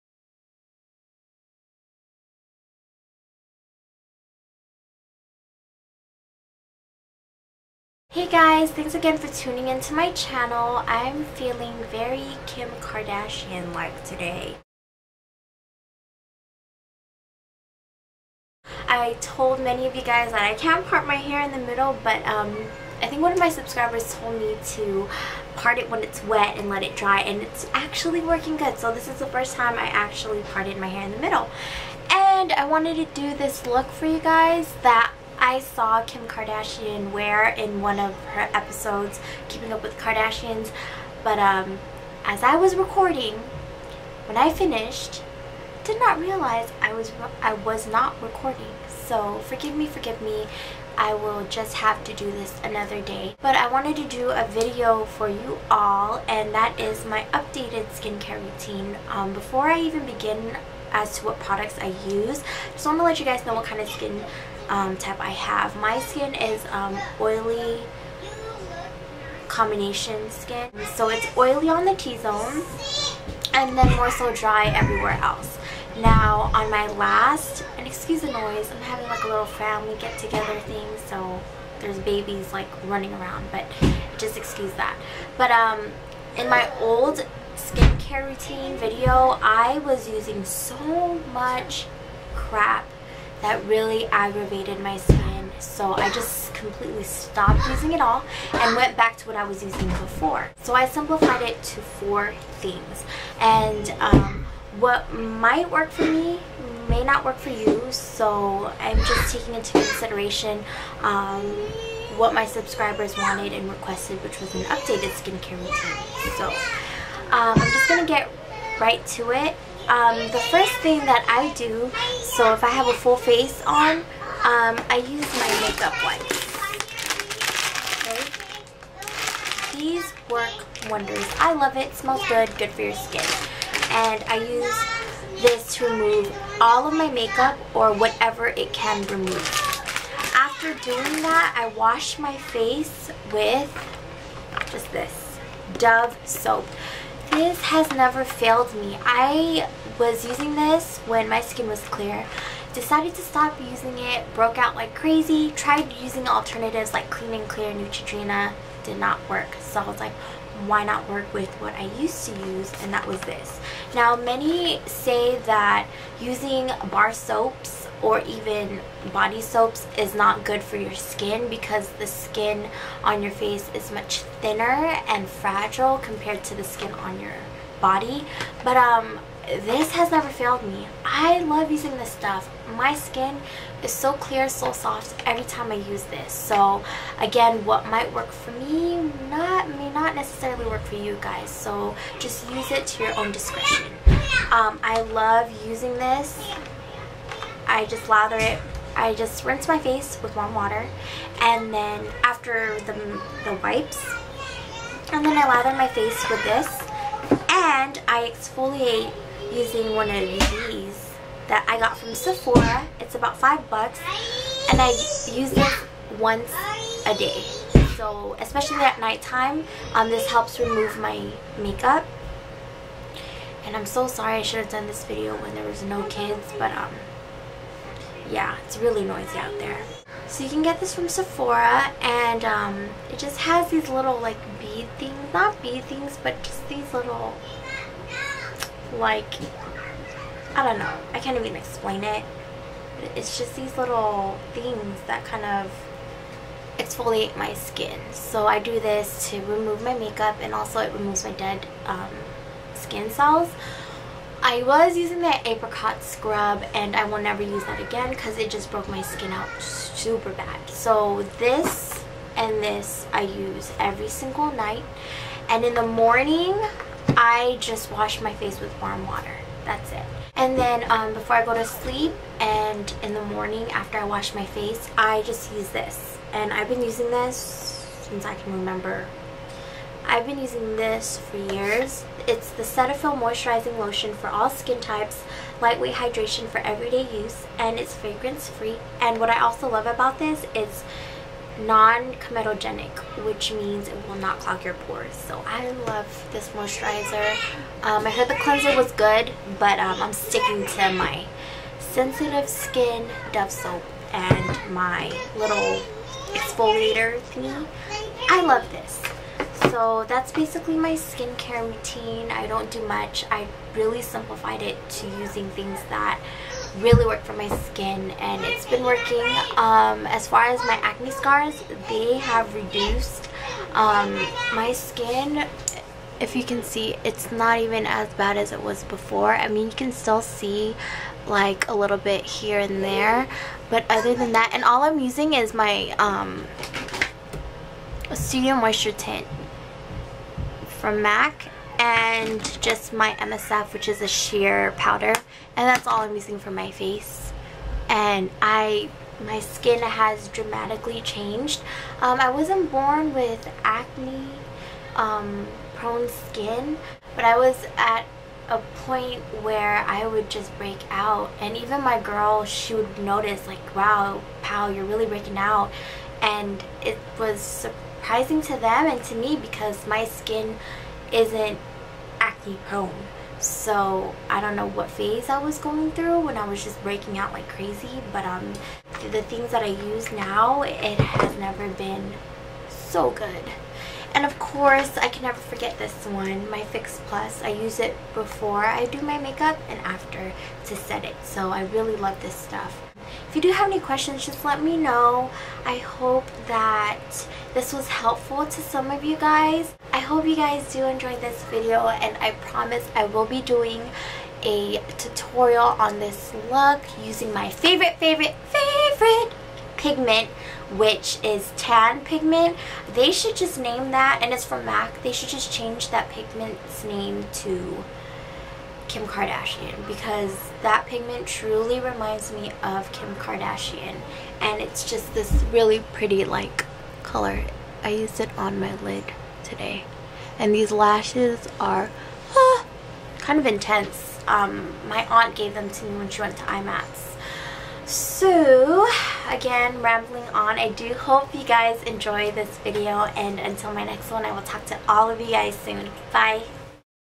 My Updated Skincare Routine | baylovespow
Here is my updated skincare routine!

PRODUCTS MENTIONED
M.A.C Cosmetics makeup wipes
Dove bar soap
Sephora exfoliating scrubber
Cetaphil moisturizing lotion